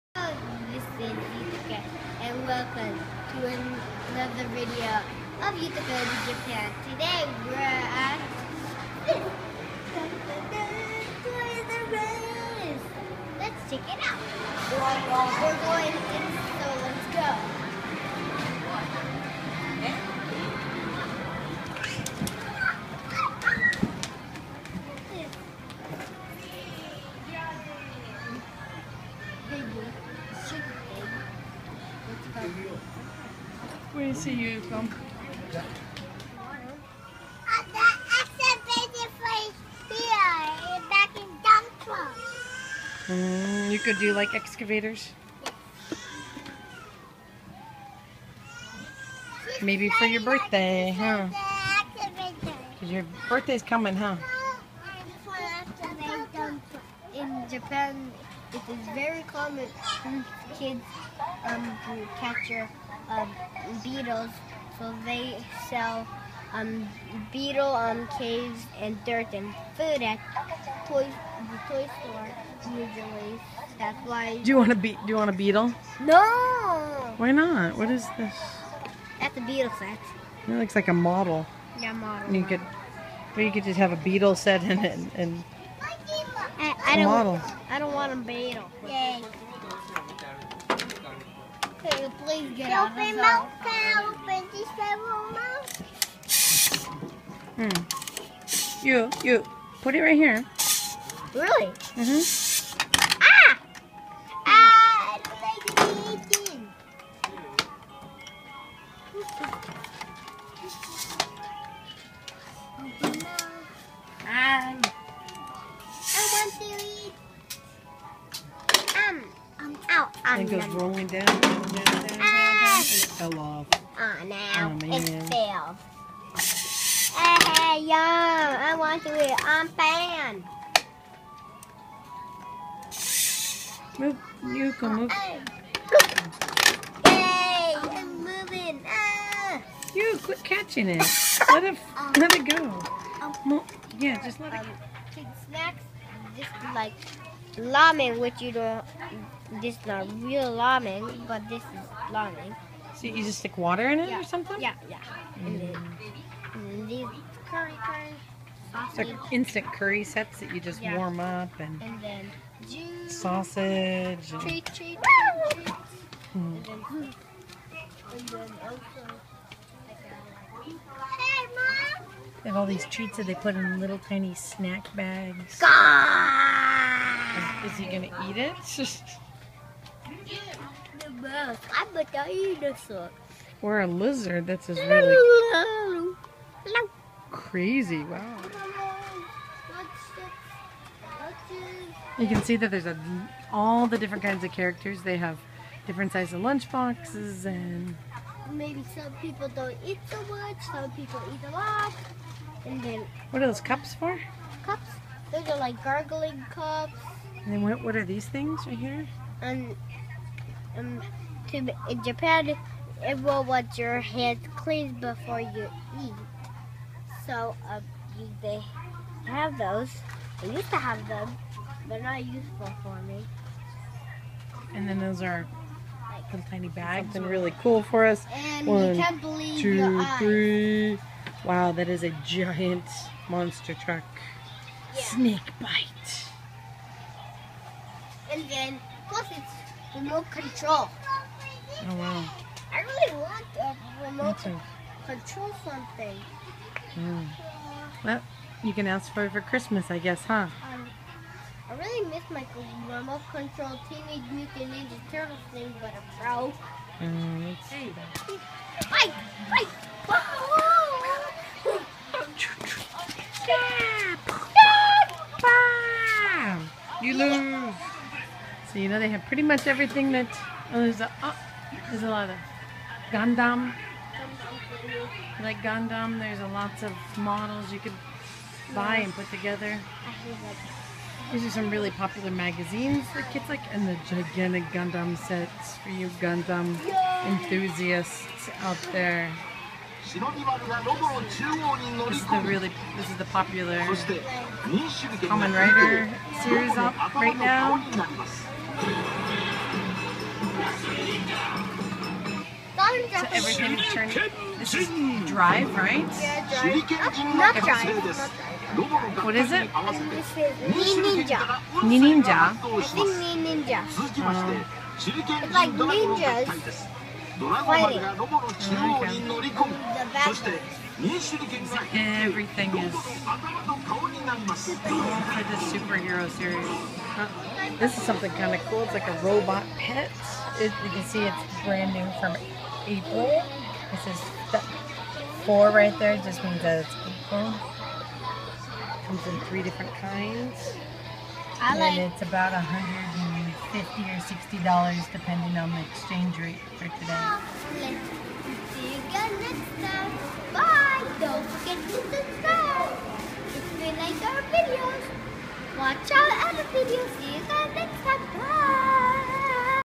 Toys RUS in Japan
Yutaka goes to Toys RUS to see what they have.